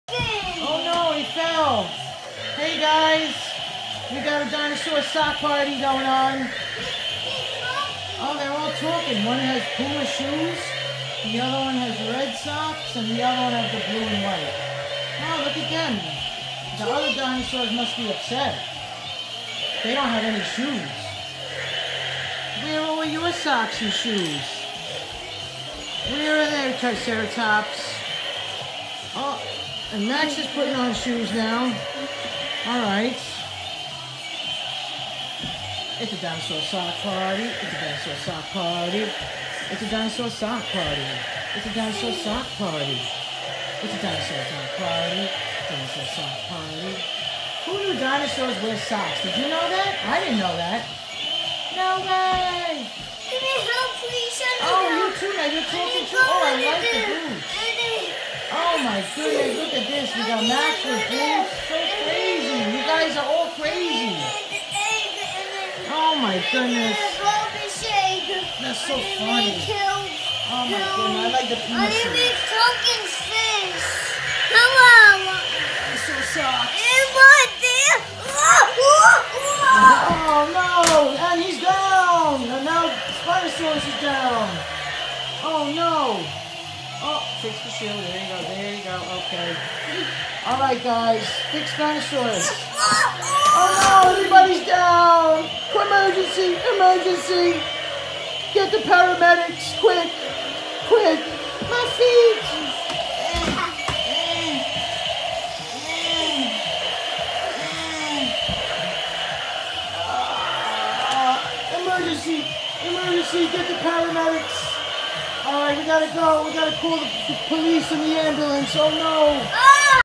MIGHTY MAXIMUS DINO TOYS DANCE IN HIS SOCKS
Walking Dino’s wearing socks dancing w max